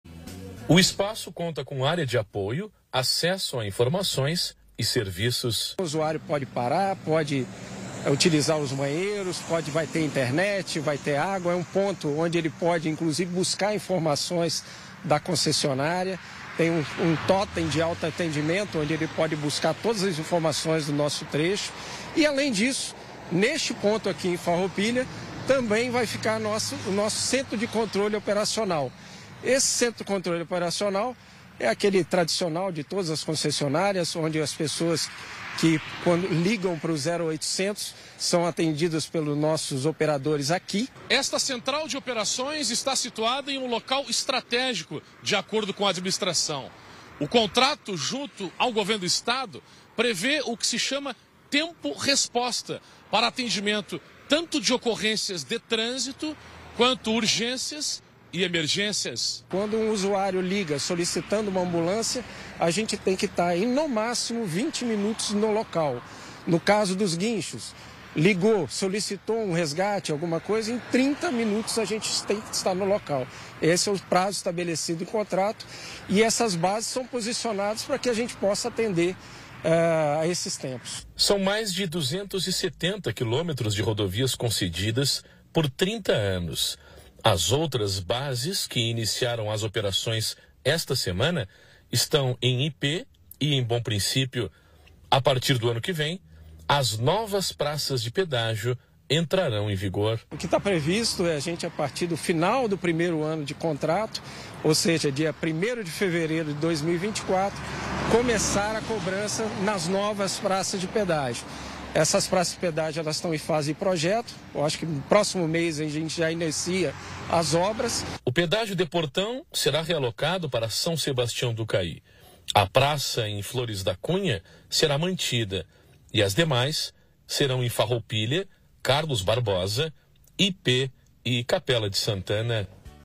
Serra: rodovias contam com base de atendimento
Motoristas da serra contam com base de atendimento nas rodovias da região.